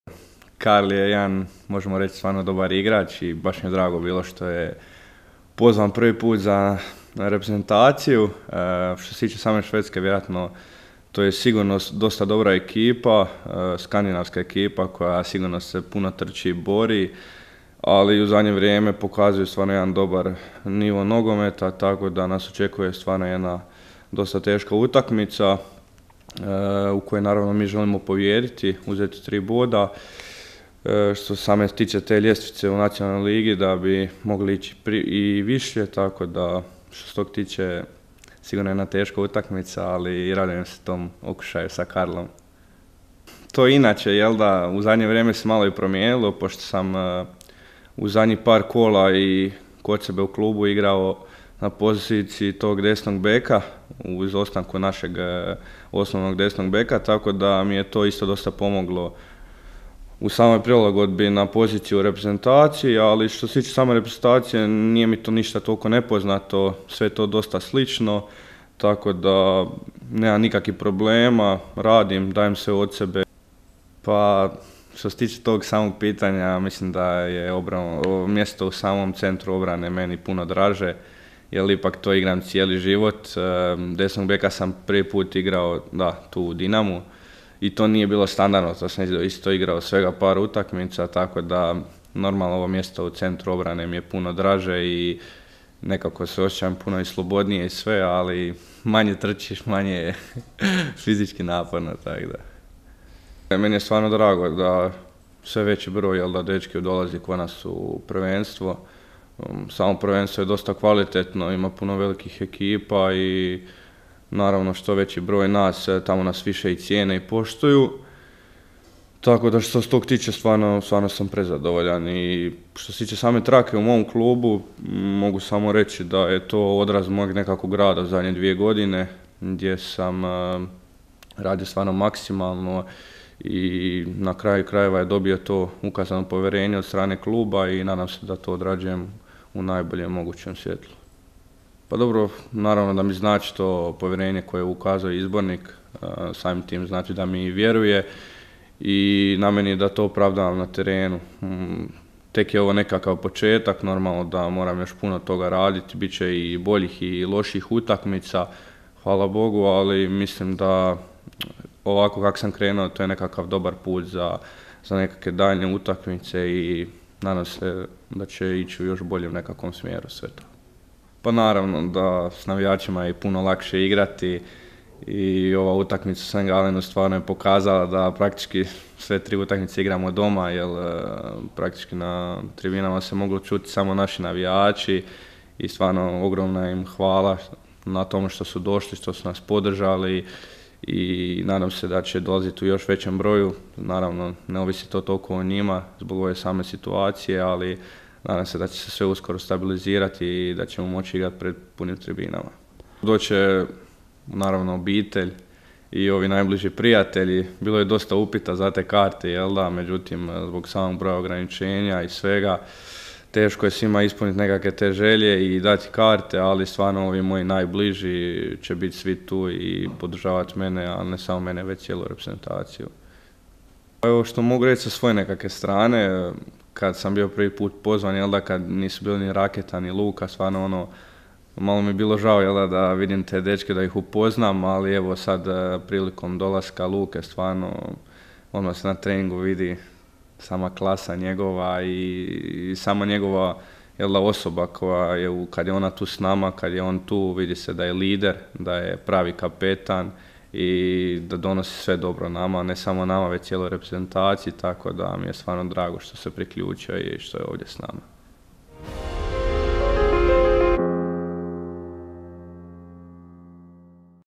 Filip Uremović uoči Švedske
Filip Uremović najavljuje skorašnju utakmicu sa Švedskom kroz prizmu svoje nove uloge na desnom beku i povratka navijača na Maksimir